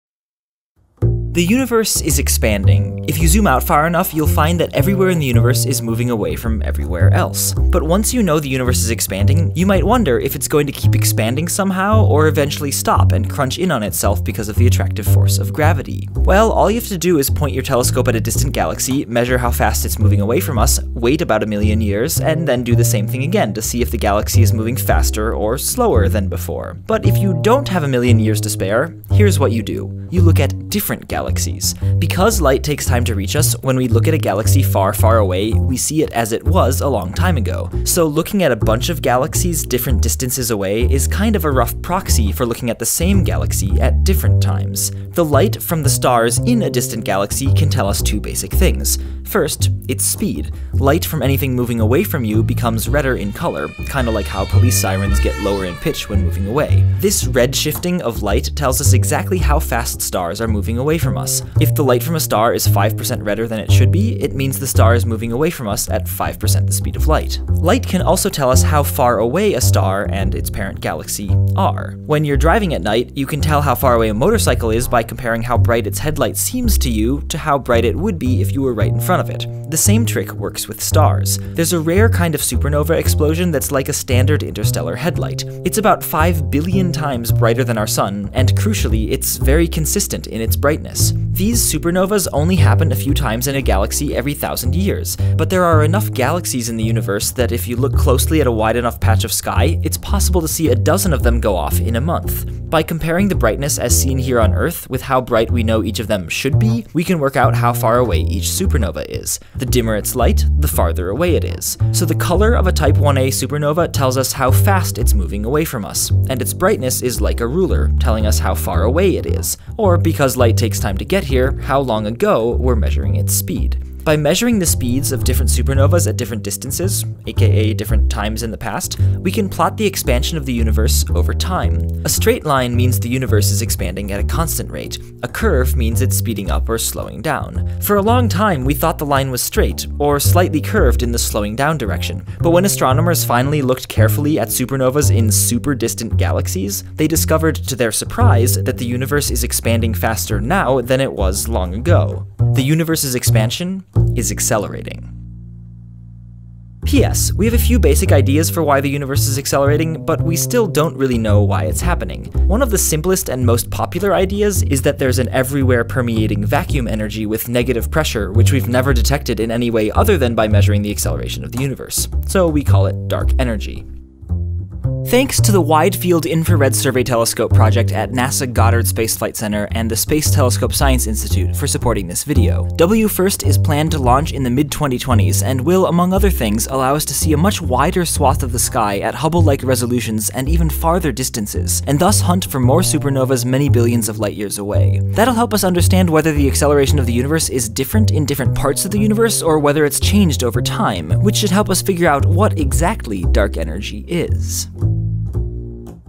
How Do We Know The Universe Is ACCELERATING?
Aatish Bhatia helped write this video! A big thanks for his help! Here's his blog

The universe is expanding – this we know from looking at red shifts of distant galaxies – but the acceleration of the universe's expansion is harder to measure. It requires measuring the change of recession velocity over time, and it's done by comparing Type Ia supernovas as standard candles at different distances, which is how we know the universe is accelerating!

REFERENCES

Straightforward references:
Ryan Foley talking about WFIRST & Dark Energy
Why does dark energy make the universe accelerate?

Slightly-technical references:
Permlutter article (old article but quite well explained & goes into future directions near the end)

Technical references:
Mukhanov’s Physical Cosmology